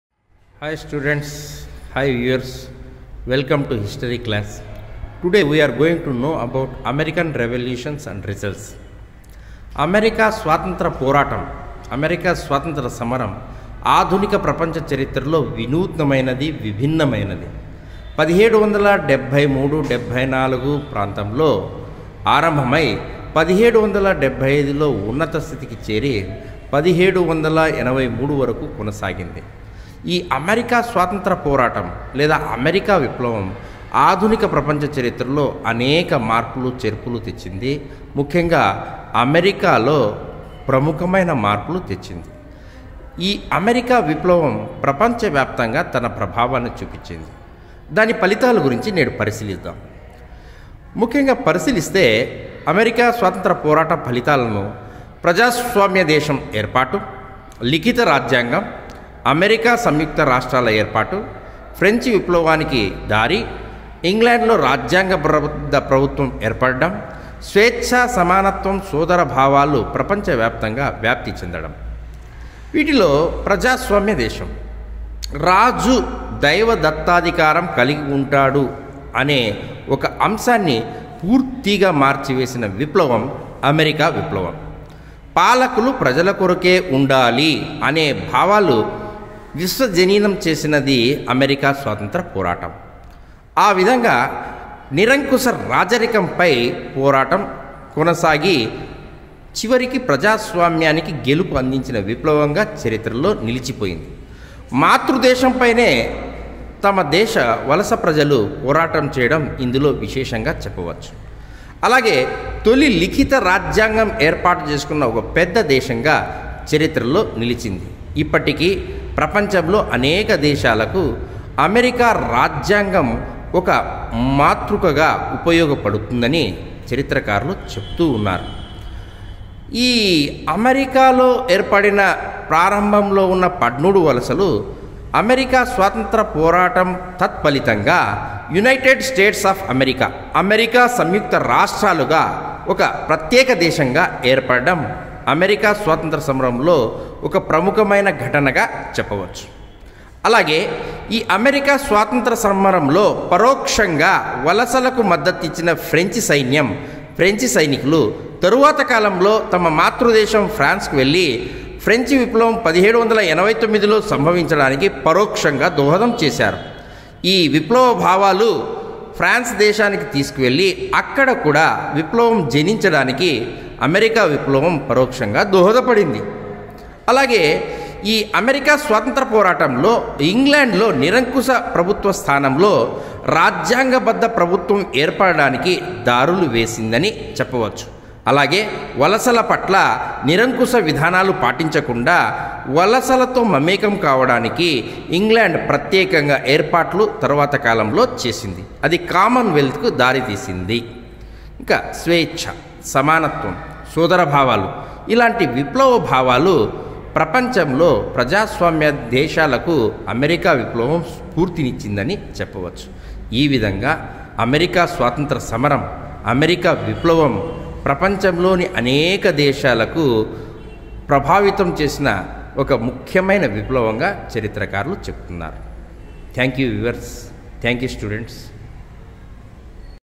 American Revolution Results-Dr D Sahadevudu
American war of independence brought many different types of results in World history.